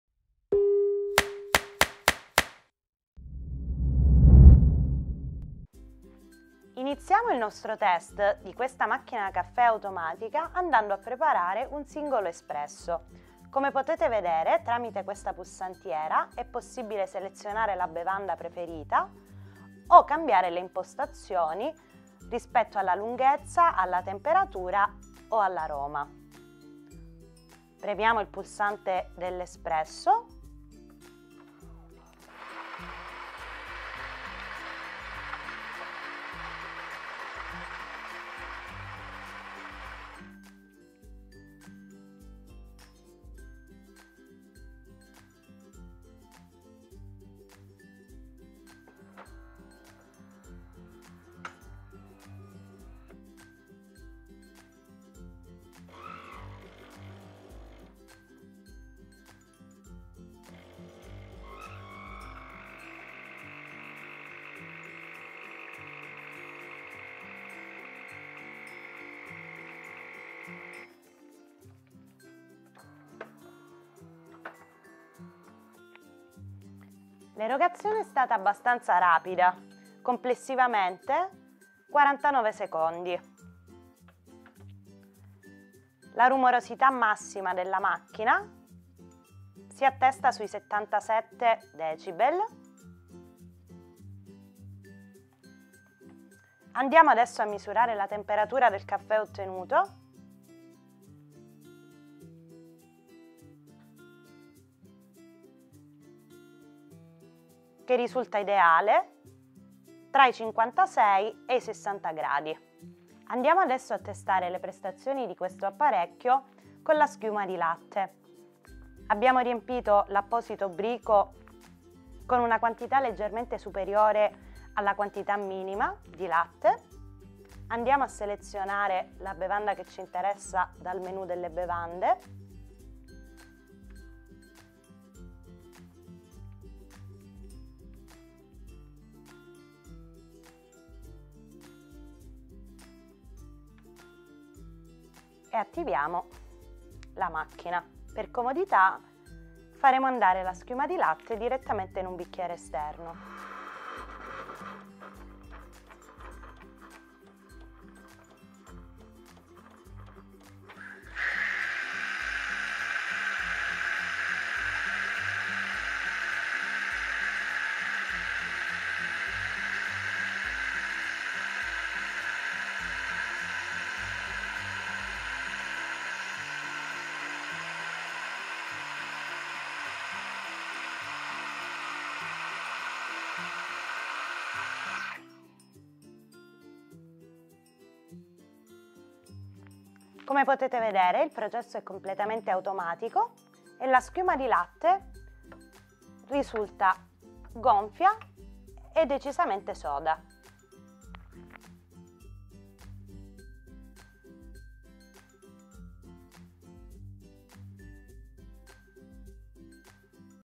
Saeco Picobaristo SM5460/10 la nostra prova | QualeScegliere.it
🔻  🔻  Clicca qui sotto per ulteriori informazioni 🔻  🔻

In questo video potete vedere la macchina da caffè automatica Saeco Picobaristo SM5460/10 in azione.
Consultate la nostra recensione completa se volete scoprire tutte le caratteristiche di questo modello.

Avete dubbi, domande, richieste per un prossimo video?

Grazie per averci seguiti, alla prossima!

Rimaniamo in contatto!